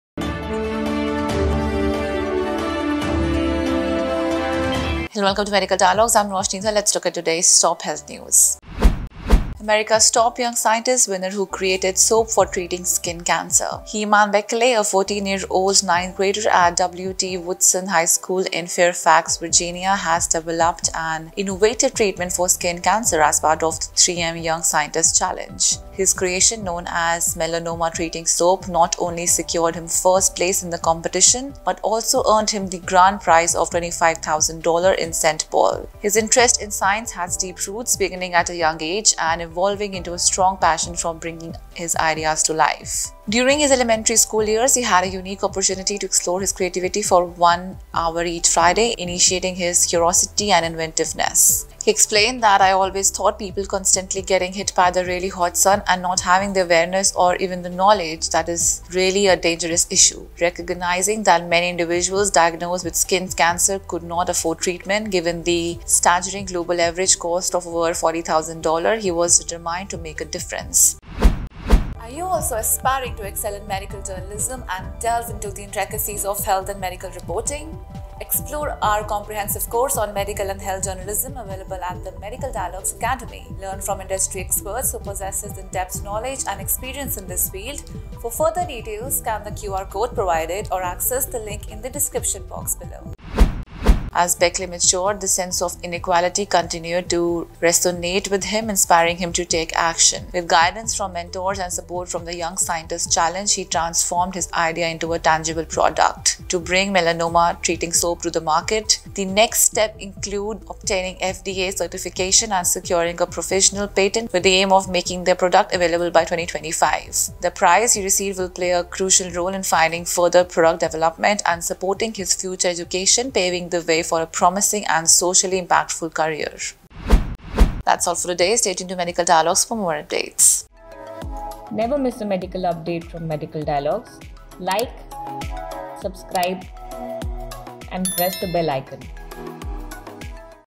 America's Top Young Scientist winner who created soap for treating skin cancer
Heman Bekele, a 14-year-old ninth grader at W.T. Woodson High School in Fairfax, Virginia, has developed an innovative treatment for skin cancer as part of the 3M Young Scientist Challenge. His creation, known as Melanoma Treating Soap, not only secured him first place in the competition but also earned him the grand prize of $25,000 in St. Paul.Bekele's interest in science has deep roots, beginning at a young age and evolving into a strong passion for bringing his ideas to life. During his elementary school years, he had a unique opportunity to explore his creativity for one hour each Friday, igniting his curiosity and inventiveness.